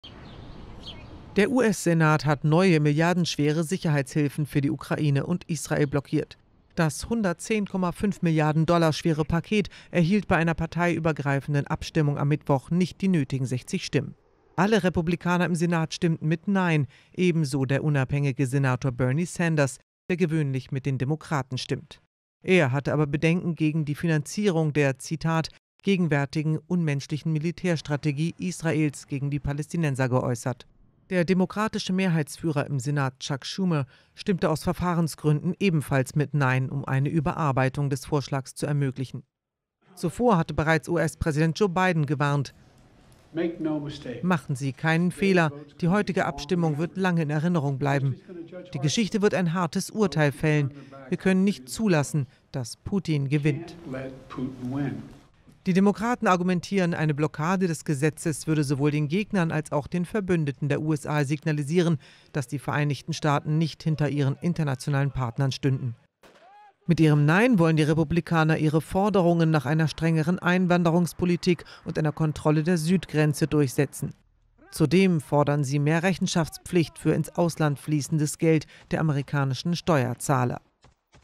US-Senat: Republikaner blockieren Hilfen für Ukraine und Israel
Die Republikaner wollen damit ihre Forderungen nach einer strengeren Einwanderungspolitik  durchsetzen. Zudem fordern sie mehr Rechenschaftspflicht für ins Ausland fließendes Geld der amerikanischen Steuerzahler.